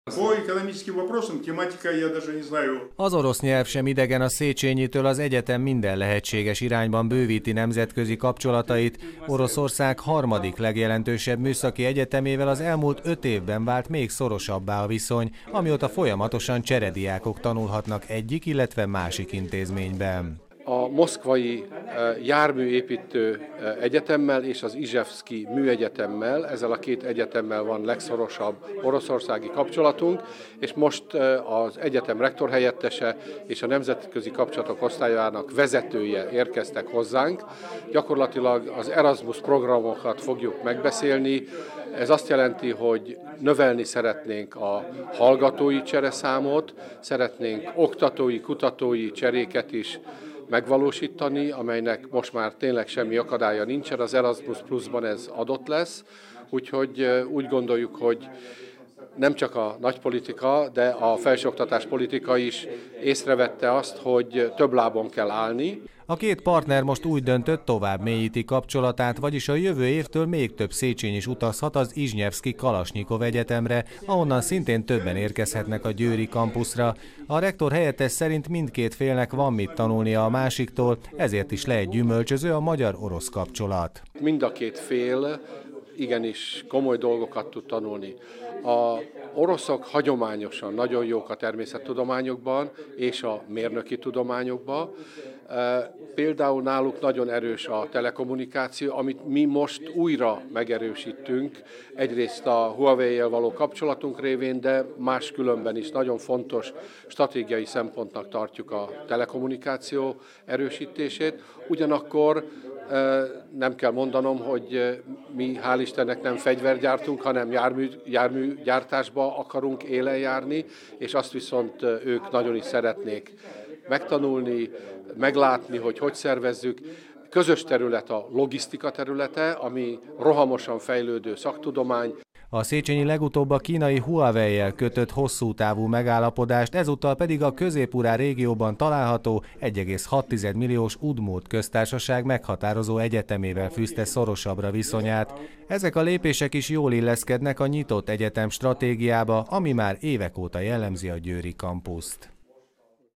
Erősödő orosz kapcsolat
Továbberősíti orosz kapcsolatait a Széchenyi István Egyetem. Az intézmény már másfél évtizede áll kapcsolatban Oroszország harmadik legnagyobb műszaki egyetemével, néhány éve pedig új lendületet vett a Széchenyi és az izsnyevszki Kalasnyikov Egyetem együttműködése. A felek most, úgy döntöttek, bővítik kapcsolatrendszerüket, így a jövőben még több cserediák tanulhat majd a meghatározó orosz intézményben és Győrbe is többen jöhetnek Oroszországból. A Széchenyi rektorhelyettese szerint mindkét félnek van mit tanulnia a másiktól.